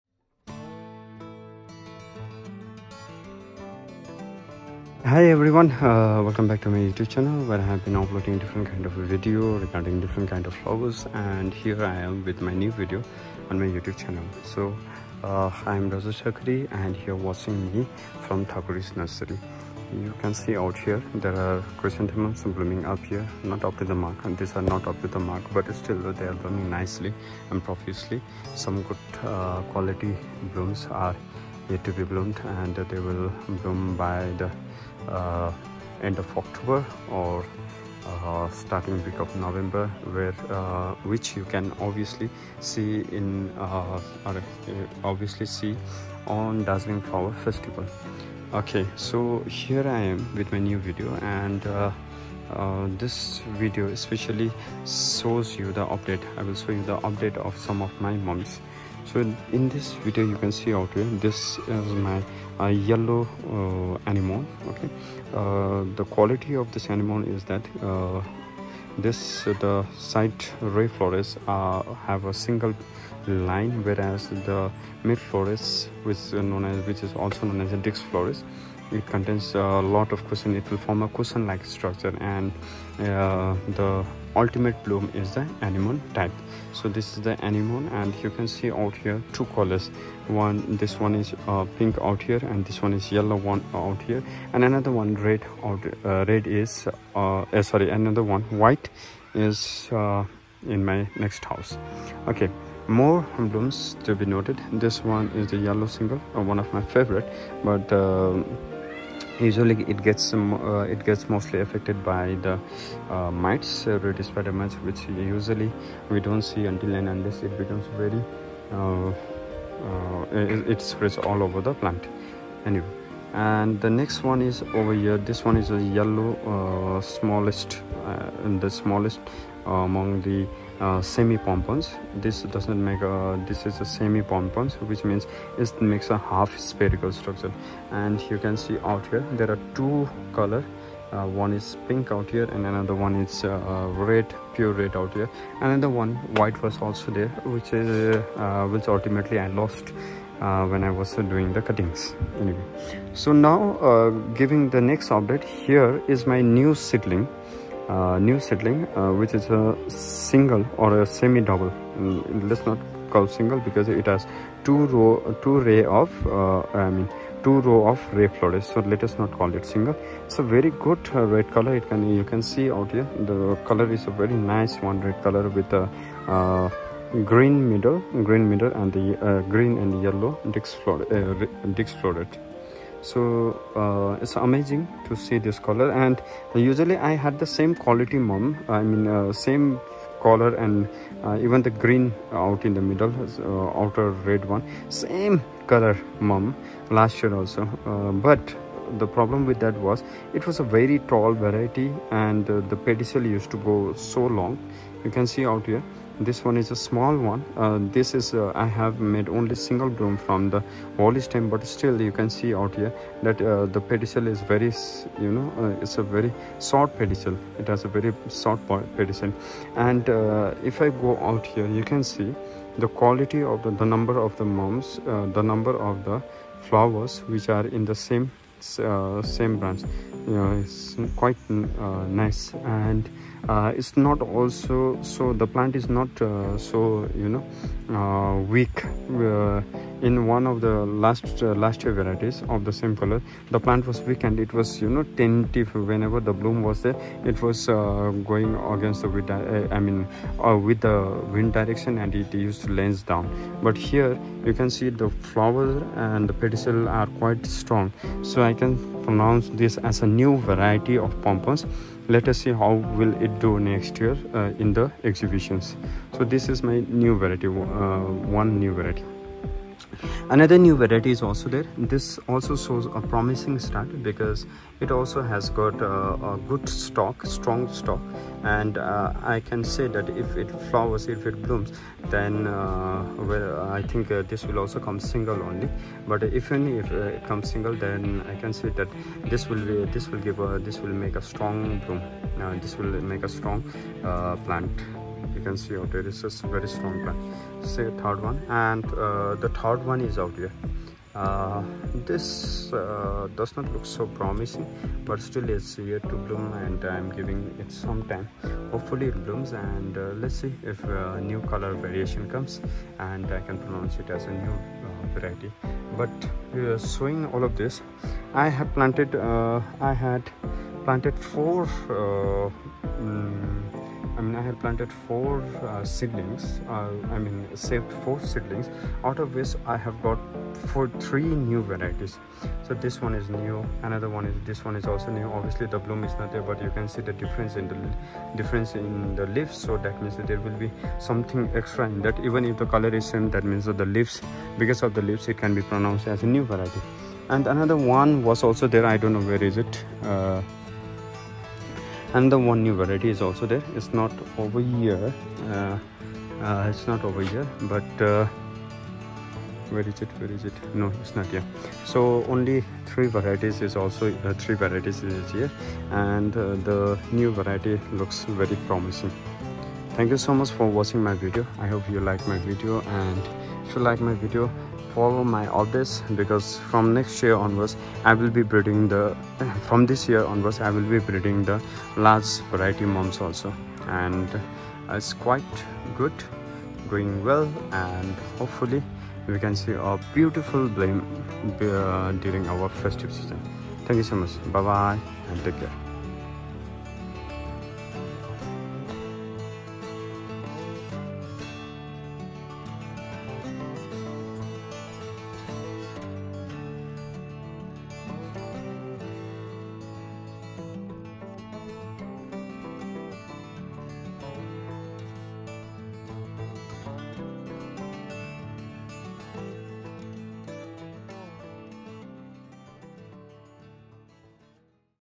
chrysanthemum chrysanthemumflower chrysanthemumplant breeding gardening flower plants love
This Video features about the new development of a class: Single mum variety.￼ The old variety of this man had weak stem, long petals(ray florets)and due to its weak stem, it was unable to hold the weight of the flowers.￼
The new development shows the shorter petals (Ray floret), strong pedicel and Stem, and much better bushy short growth.

Hope it will do good in our next year exhibition.